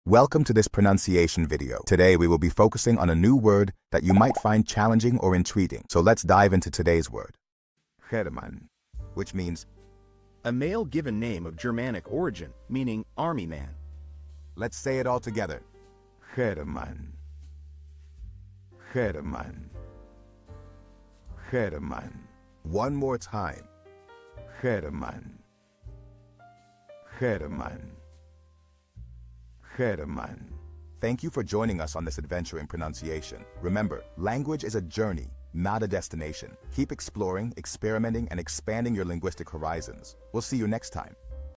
How to pronounce Herman
Master the Pronunciation of 'Herman WHICH MEANS HERMAN' - which means : A Male Given Name of Germanic Origin, Meaning "Army Man." 📝 with @PronunciationSpanish 🎓🗣️ - [Your Guide to Spanish]

Learn to pronounce 'Herman' perfectly in Spanish with our easy and engaging tutorial by @PronunciationSpanish! 📖📽️ In this comprehensive video, we provide you with native speaker examples 🗣️👂 and phonetic breakdowns 🎵 to ensure you get the pronunciation just right.

How to say Herman in Spanish